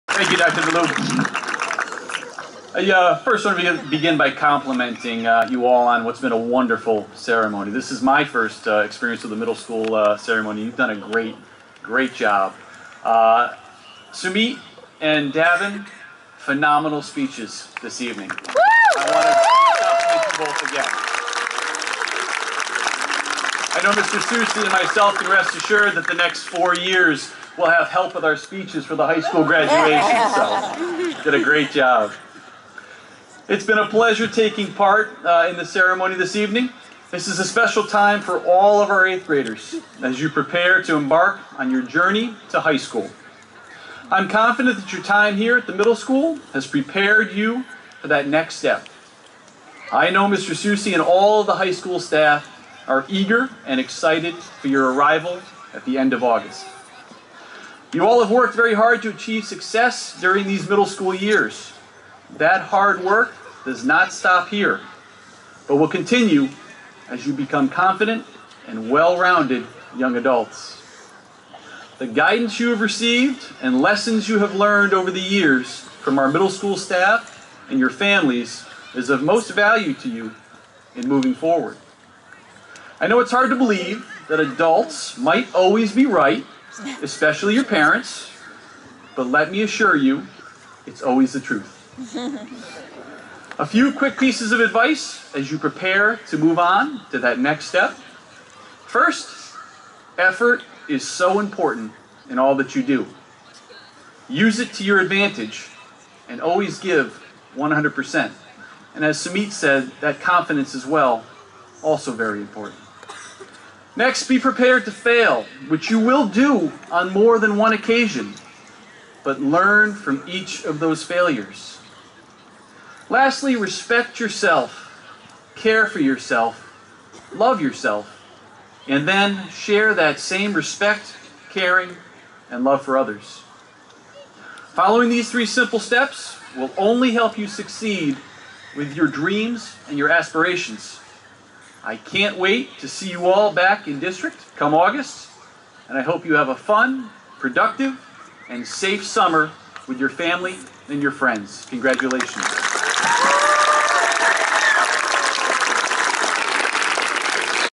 3 Jeffery Newton
East Lyme Middle School Graduation 2015  Jeffery Newton